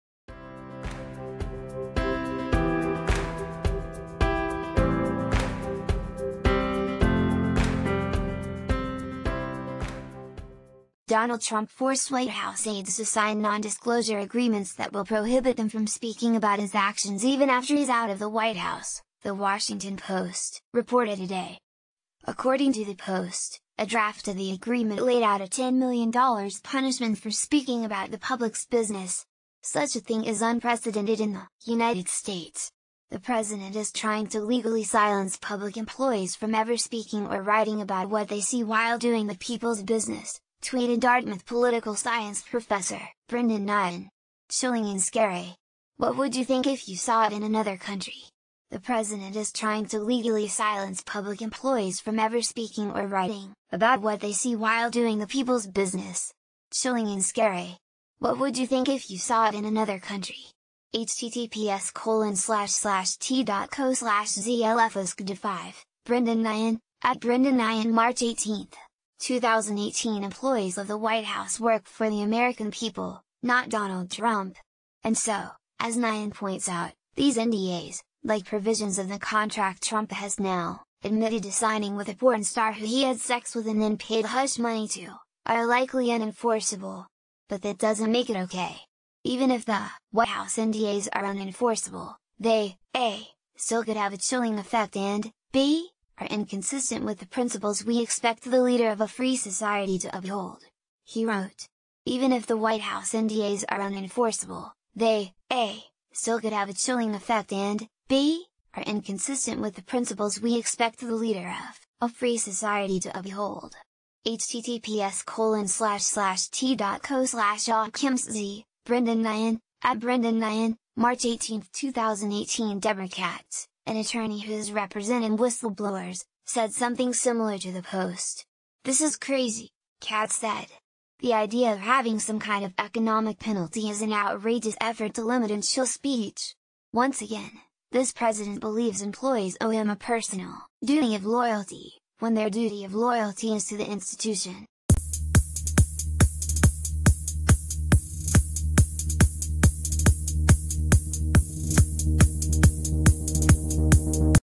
[News] ‘Chilling and scary:’ Ivy league prof rips Trump’s unprecedented White House nondisclosure...
[News] ‘Chilling and scary:’ Ivy league prof rips Trump’s unprecedented White House nondisclosure agreements
Donald Trump forced White House aides to sign nondisclosure agreements that will prohibit them from ...